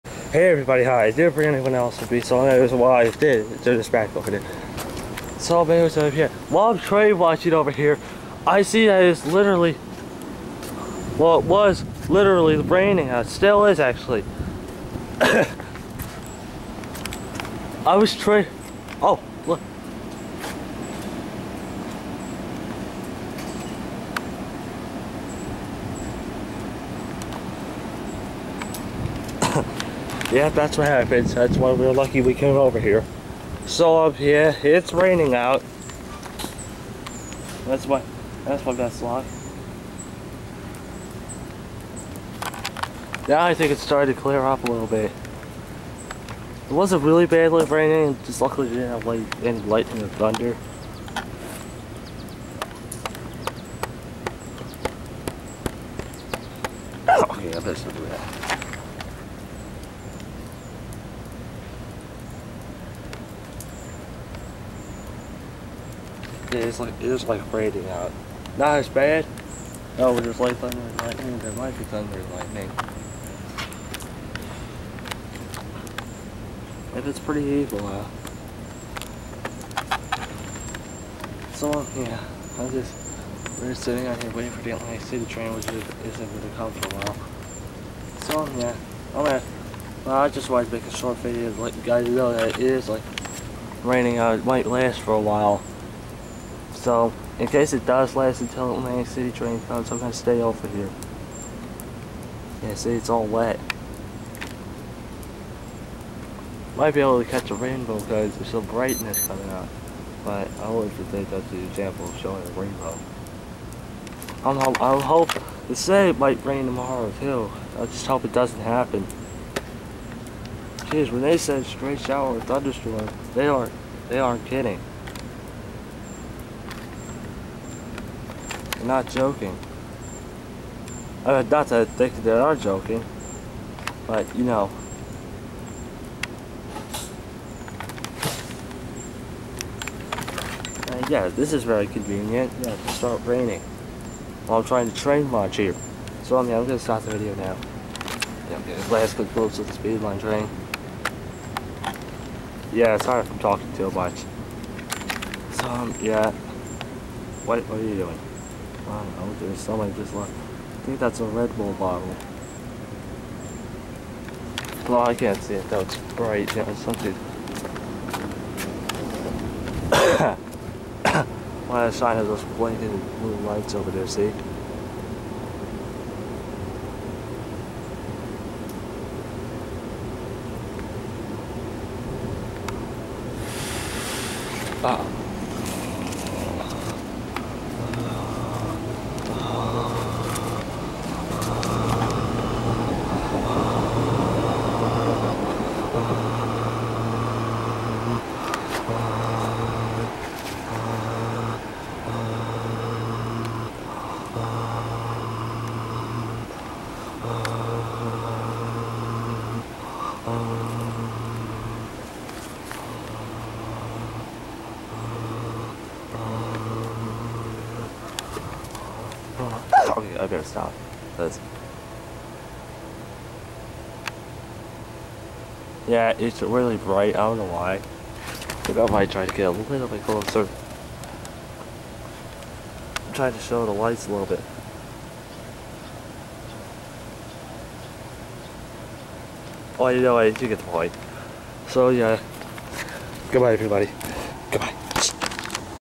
After the rainstorm.
Once again, I apologize for the late upload. Anyways while I was waiting for the trains, I explain on what happened, just watch the video.